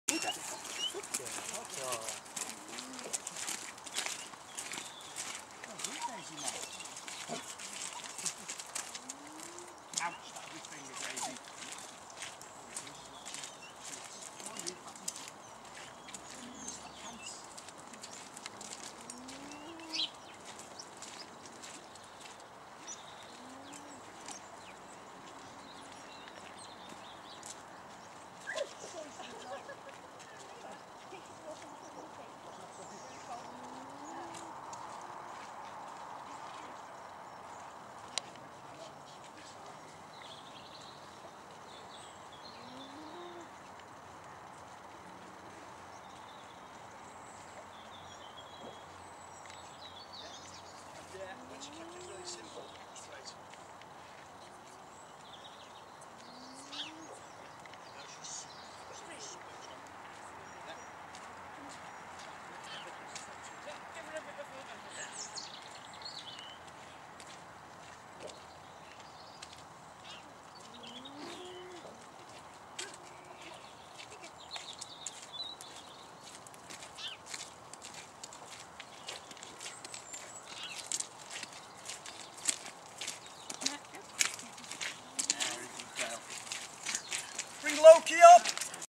Educating Alice
The last pup to work with Daisy was Tikka, who is generally the most confident. (Don't let the fat bloke step on your pup).